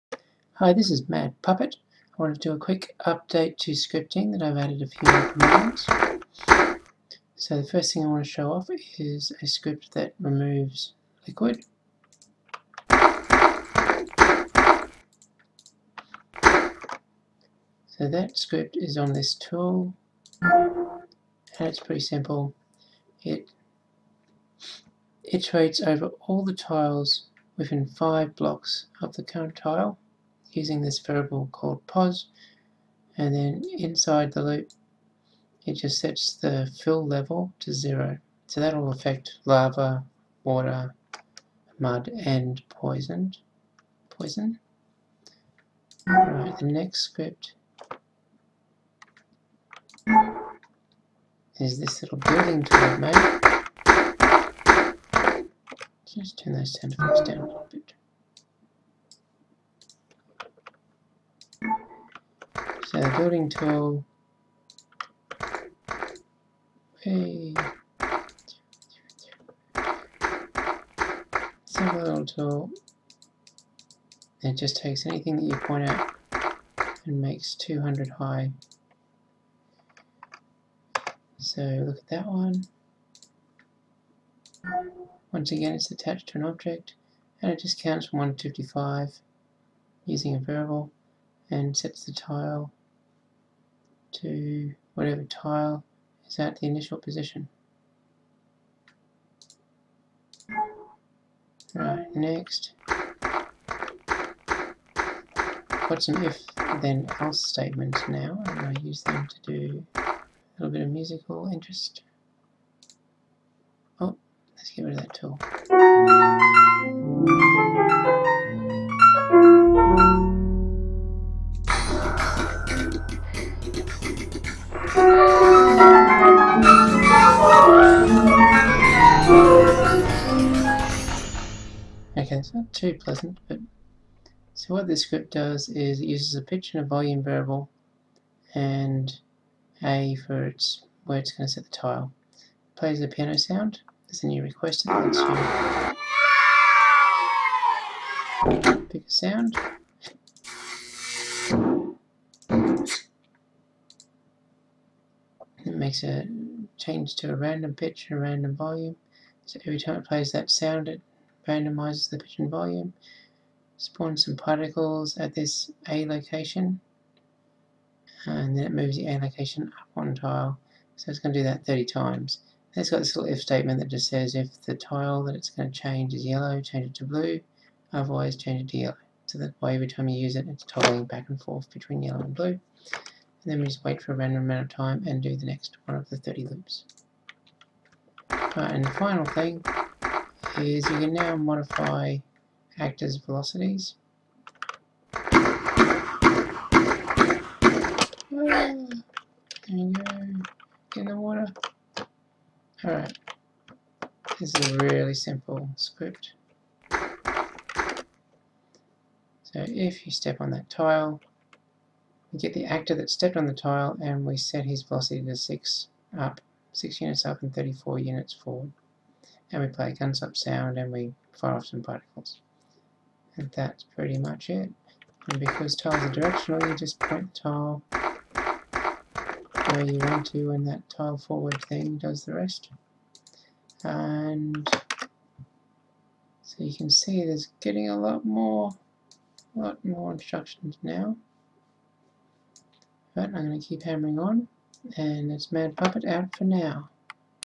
Buildville Scripting Preview 3
Another update on the visual scripting progress... some more commands

- if then else statements
- better dialogs for sound and effects selection
- affect player velocity now
- change tile fill level